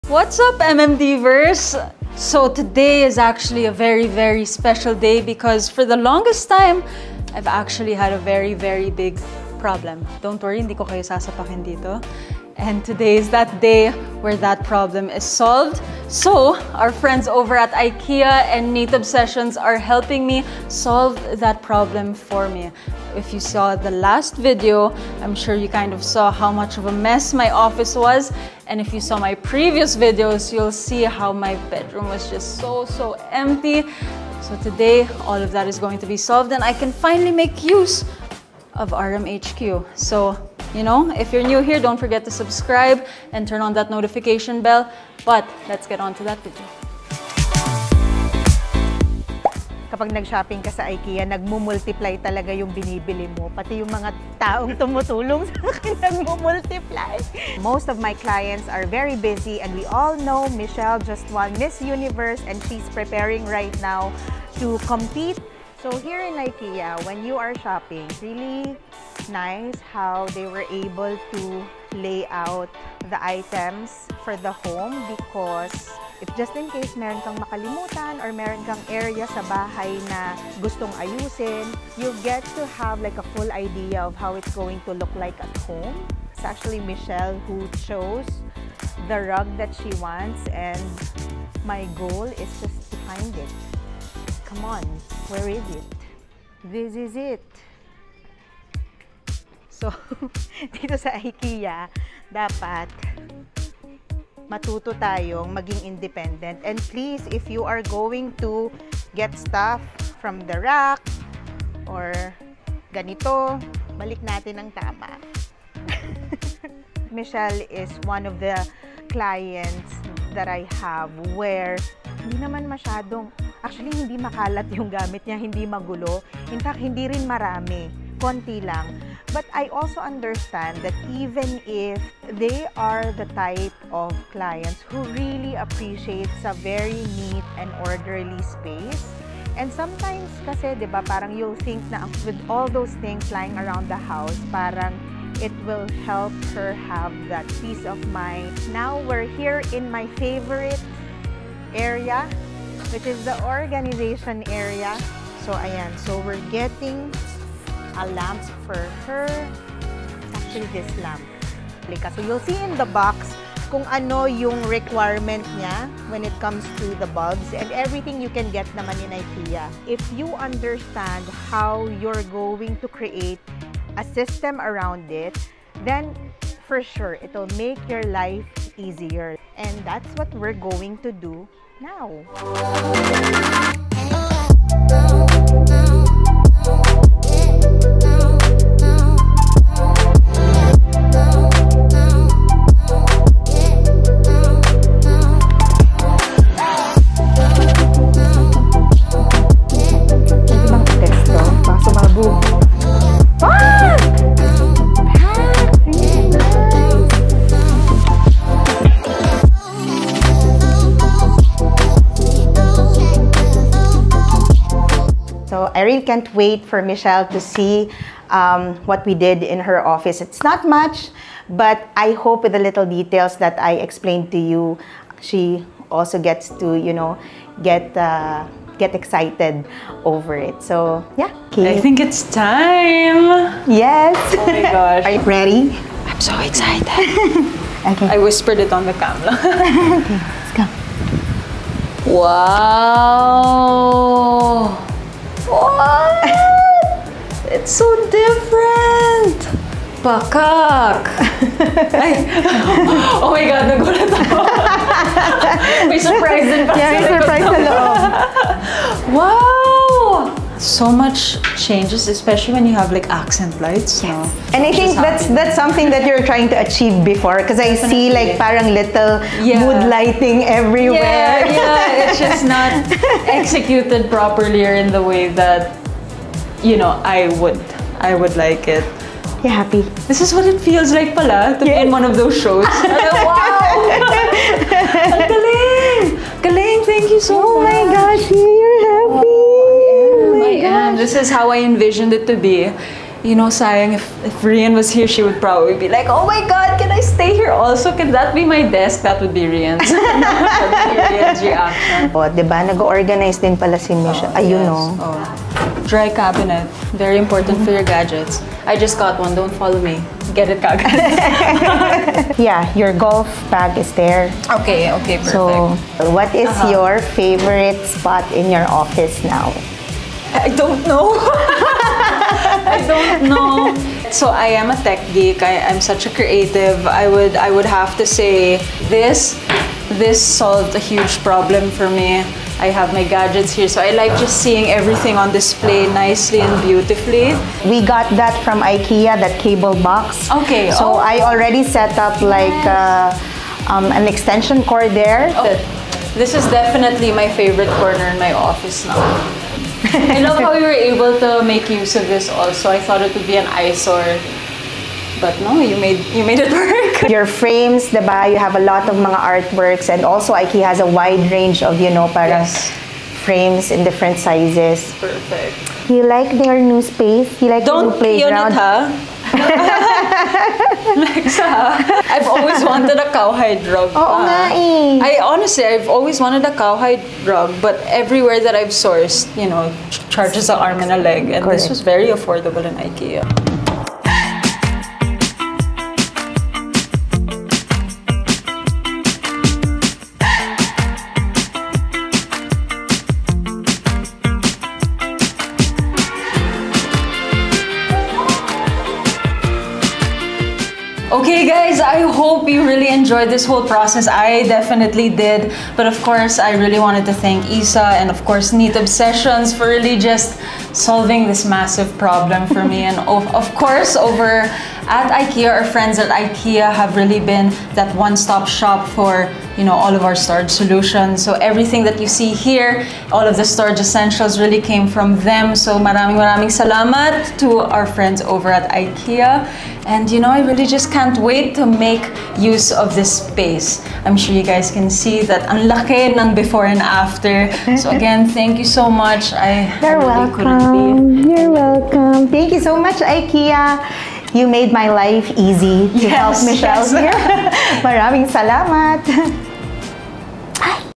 HOME TRANSFORMATION (FIT FOR A QUEEN) | ft. RHIMICH HQ, Michelle Dee, Neat Obsessions, IKEA
Hey MMDVERSE!

Here’s a long overdue RHIMICH HQ aka “RMHQ” video we (yes we) have all been waiting for!

Watch how NEAT OBSESSIONS helped me transform my “organized cluttered” life into an organization heaven using products from the BEST in storage solutions - @IKEAPhilippinesOfficial 👑

I’d of course love to thank the wifey @whianwamos for hyping up @NeatObsessions and vouchinh for their work ❤️‍🔥 all love to their whole team